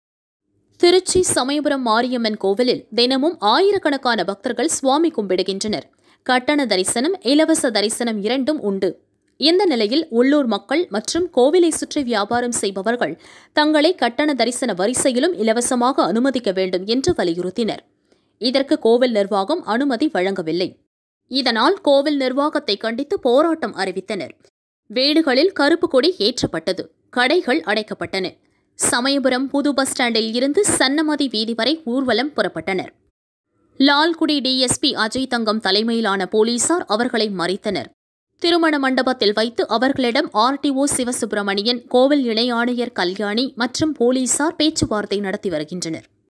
சமயபுரம் கோயிலில் இலவச தரிசனம் கேட்டு உள்ளூர் மக்கள் போராட்டம் | samayapuram mariamman kovil
சமயபுரம் கோயிலில் இலவச தரிசனம் கேட்டு உள்ளூர் மக்கள் போராட்டம் | samayapuram mariamman kovil#திருச்சி